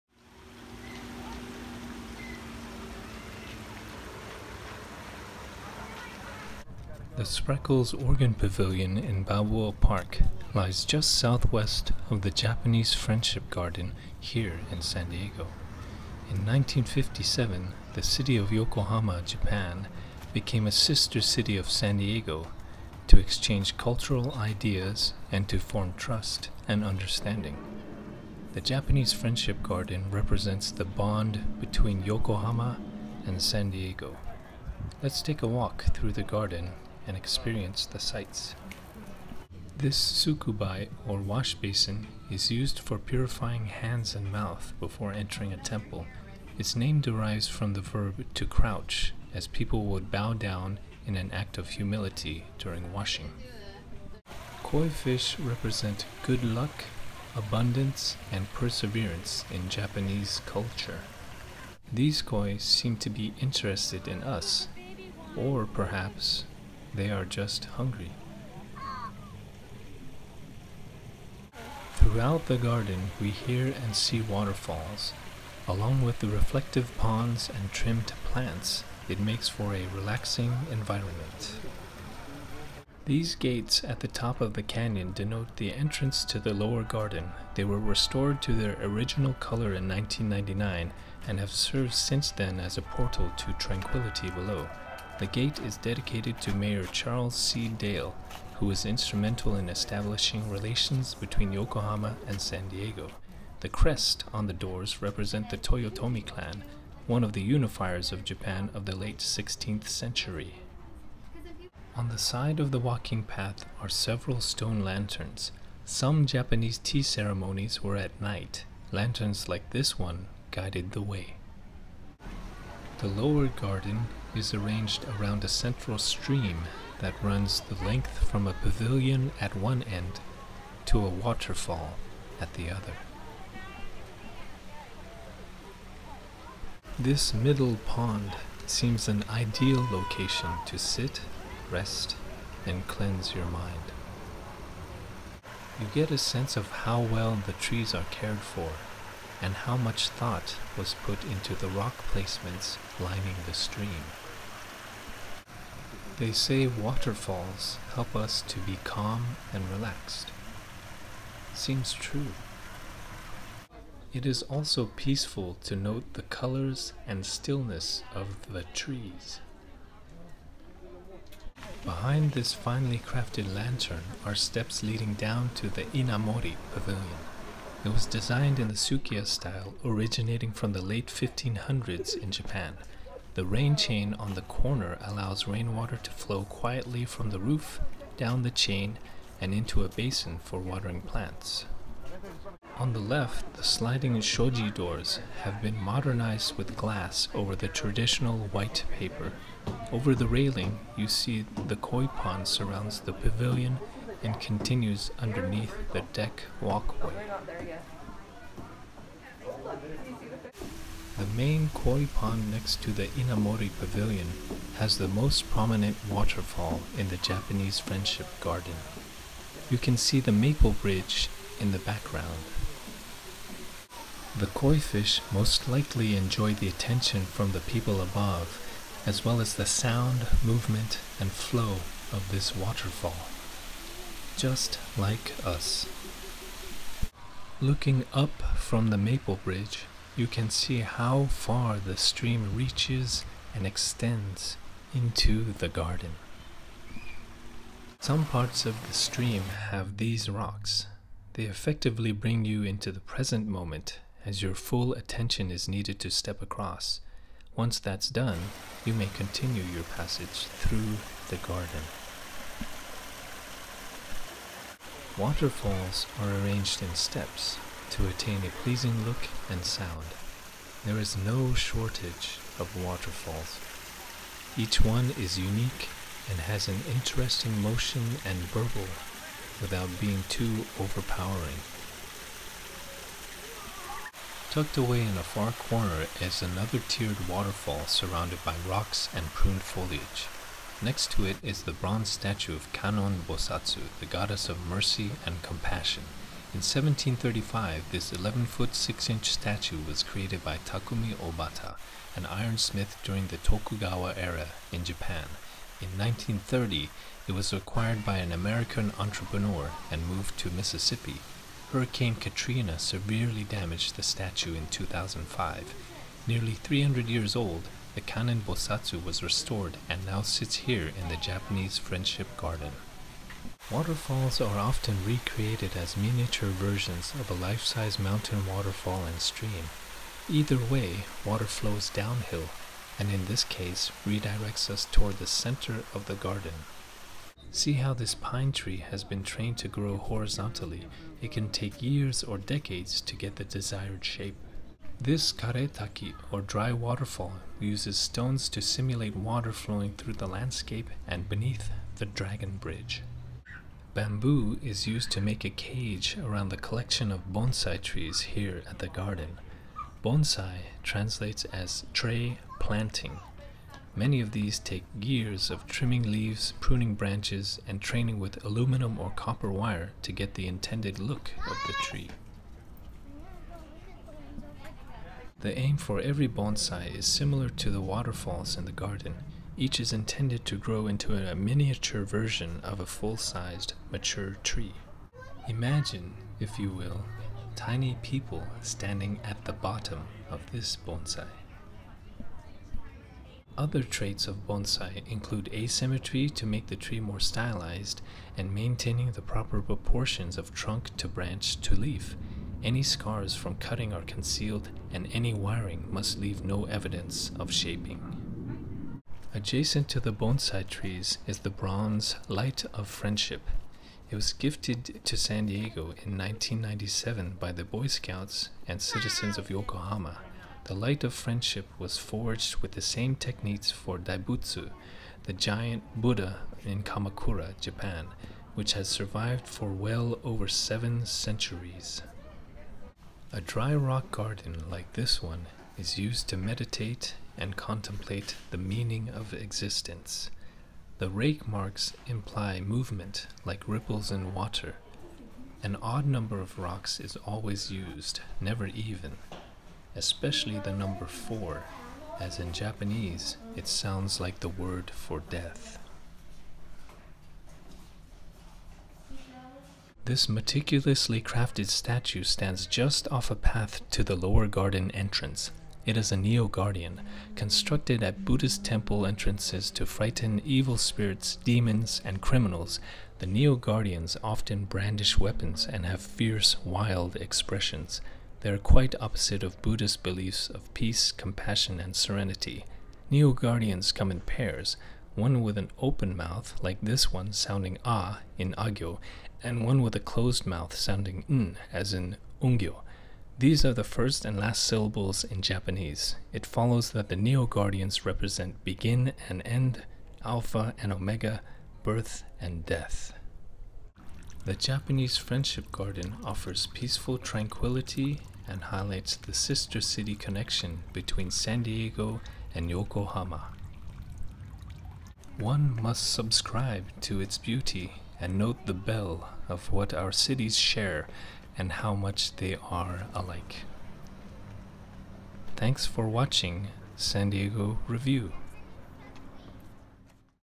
Japanese Friendship Garden | San Diego Review
Located in Balboa Park at 2215 Pan American Rd E, San Diego, CA 92101
The Spreckels Organ Pavilion in Balboa Park lies just southwest of the Japanese Friendship Garden here in San Diego.
In 1957, the City of Yokohama, Japan became a Sister City of San Diego, to exchange cultural ideas and to form trust and understanding.
The Japanese Friendship Garden represents the bond between Yokohama and San Diego.
Let's take a walk through the Garden and experience the sights.
This tsukubai, or washbasin, is used for purifying hands and mouth before entering a temple. Its name derives from the verb "to crouch" as people would bow down in an act of humility during washing.
Koi fish represent good luck, abundance, and perseverance in Japanese culture.
These Koi seem to be interested in us, or perhaps they are just hungry.
Throughout the Garden we hear and see waterfalls. Along with the reflective ponds and trimmed plants, it makes for a relaxing environment.
These gates at the top of the canyon denote the entrance to the lower garden. They were restored to their original color in 1999 and have served since then as a portal to tranquility below. The gate is dedicated to Mayor Charles C. Dail who was instrumental in establishing relations between Yokohama and San Diego.
The crest on the doors represent the Toyotomi Clan, one of the "Unifiers of Japan" of the late 16th century.
On the side of the walking path are several stone lanterns. Some Japanese tea ceremonies were at night, lanterns like this one guided the way.
The lower garden is arranged around a central stream that runs the length from a pavilion at one end to a waterfall at the other.
This middle pond seems an ideal location to sit, rest, and cleanse your mind.
You get a sense of how well the trees are cared for and how much thought was put into the rock placements lining the stream.
They say waterfalls help us to be calm and relaxed. Seems true.
It is also peaceful to note the colors and stillness of the trees.
Behind this finely crafted lantern are steps leading down to the Inamori Pavilion.
It was designed in the Sukiya style originating from the late 1500s in Japan.
The rain chain on the corner allows rainwater to flow quietly from the roof, down the chain, and into a basin for watering plants.
On the left the sliding shoji doors have been modernized with glass over the traditional white paper. Over the railing, you see the Koi pond surrounds the pavilion and continues underneath the deck walkway.
The main Koi pond next to the Inamori Pavilion has the most prominent waterfall in the Japanese Friendship Garden. You can see the Maple Bridge in the background.
The Koi fish most likely enjoy the attention from the people above as well as the sound, movement, and flow of this waterfall. Just like us.
Looking up from the Maple Bridge, you can see how far the stream reaches and extends into the garden.
Some parts of the stream have these rocks. They effectively bring you into the present moment as your full attention is needed to step across. Once that's done, you may continue your passage through the garden.
Waterfalls are arranged in steps to attain a pleasing look and sound.
There is no shortage of waterfalls. Each one is unique and has an interesting motion and burble without being too overpowering.
Tucked away in a far corner is another tiered waterfall surrounded by rocks and pruned foliage. Next to it is the bronze statue of Kannon Bosatsu, the Goddess of Mercy and Compassion. In 1735, this 11'6" statue was created by Takumi Obata, an iron smith during the Tokugawa Era in Japan. In 1930, it was acquired by an American entrepreneur and moved to Mississippi. Hurricane Katrina severely damaged the statue in 2005. Nearly 300 years old, the Kannon Bosatsu was restored and now sits here in the Japanese Friendship Garden.
Waterfalls are often re-created as miniature versions of a life-size mountain waterfall and stream. Either way water flows downhill, and in this case, redirects us toward the center of the garden.
See how this pine tree has been trained to grow horizontally. It can take years or decades to get the desired shape.
This karetaki, or dry waterfall, uses stones to simulate water flowing through the landscape and beneath the Dragon Bridge.
Bamboo is used to make a cage around the collection of bonsai trees here at the garden. Bonsai translates as "tray planting." Many of these take years of trimming leaves, pruning branches, and training with aluminum or copper wire to get the intended look of the tree.
The aim for every bonsai is similar to the waterfalls in the garden. Each is intended to grow into a miniature version of a full-sized, mature tree.
Imagine, if you will, tiny people standing at the bottom of this bonsai.
Other traits of bonsai include asymmetry to make the tree more stylized and maintaining the proper proportions of trunk to branch to leaf. Any scars from cutting are concealed and any wiring must leave no evidence of shaping.